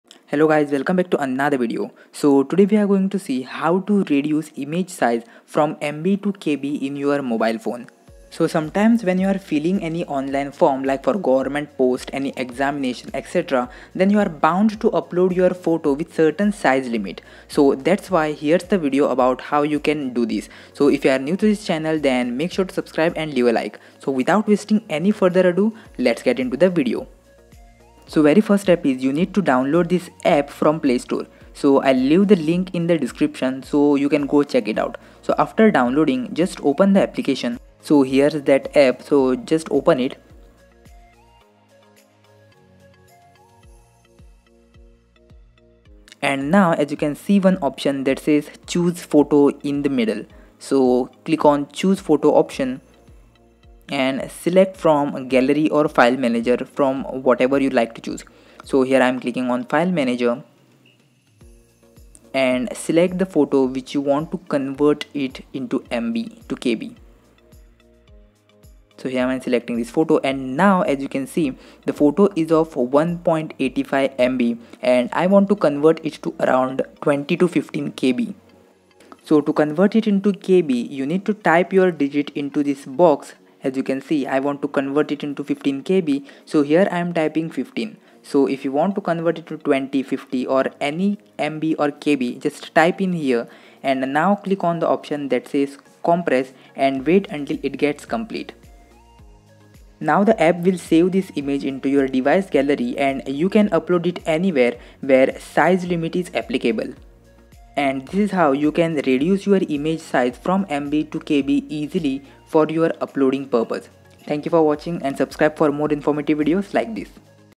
Easy Ways To Reduce Image Size In Kb On Your Android Phone
Hey guys today in this video we'll see How To Reduce Image Size In Kb In Mobile.

Sometimes you require to reduce image size to kb when uploading to govt websites, And also for other purposes if you require to reduce photo size to kb without losing quality then this video is gonna help you definitely.

So watch this video till the end to find out how you can reduce photo size from mb to kb in mobile.